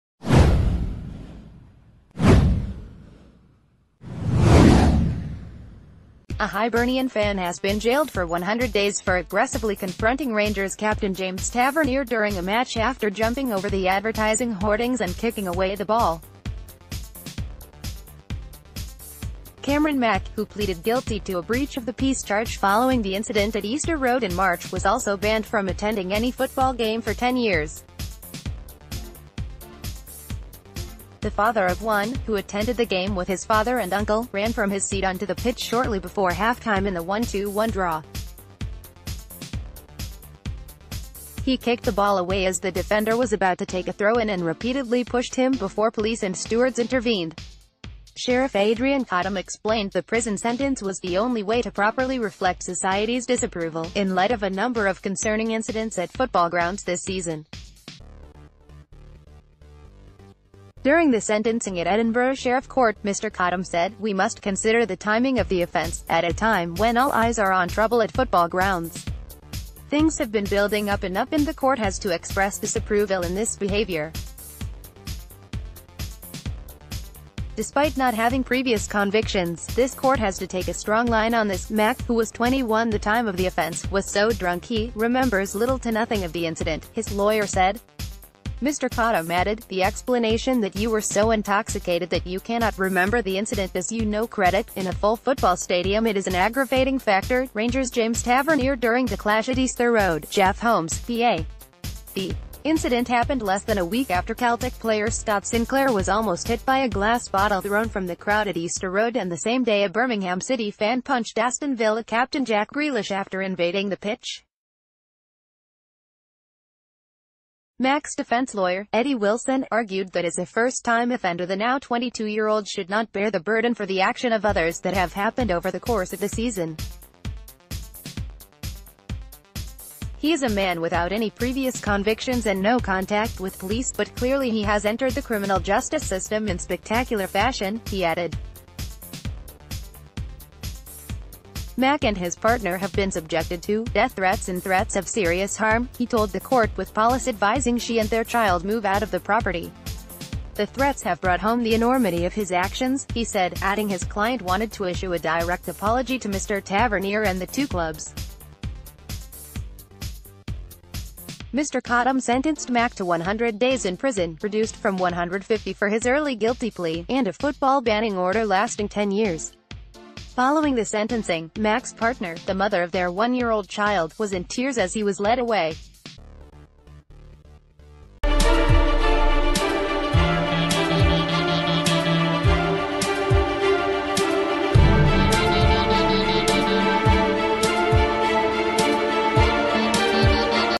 VIDEO: Hibernian fan jailed for confronting Rangers captain during match
A Hibernian fan has been jailed for 100 days for aggressively confronting Rangers captain James Tavernier during a match after jumping over the advertising hoardings and kicking away the ball. Cameron Mack, who pleaded guilty to a breach of the peace charge following the incident at Easter Road in March, was also banned from attending any football game for 10 years. The father-of-one, who attended the game with his father and uncle, ran from his seat on to the pitch shortly before half time in t...